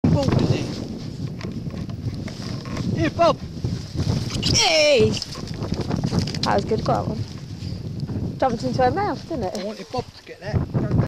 terriers ratting
this rat took the leap of faith!!!